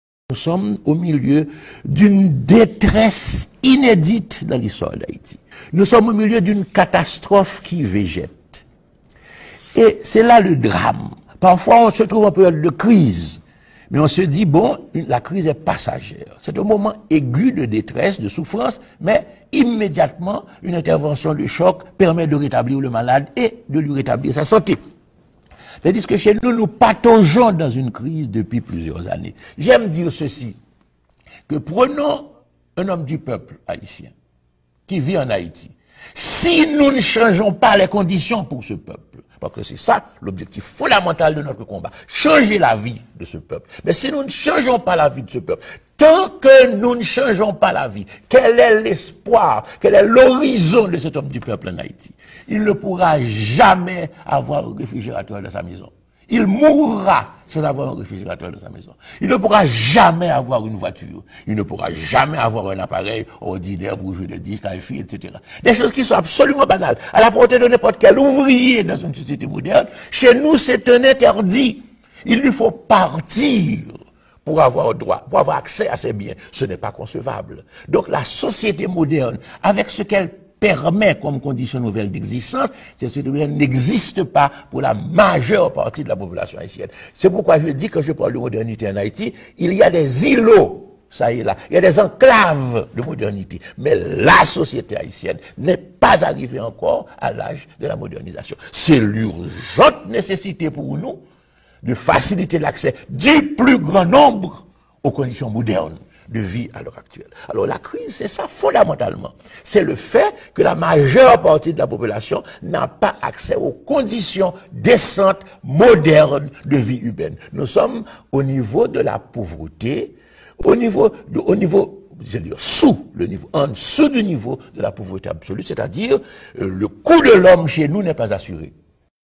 Leslie F Manigat - Parole sacrée
Nous sommes au milieu d'une détresse inédite dans l'histoire d'Haïti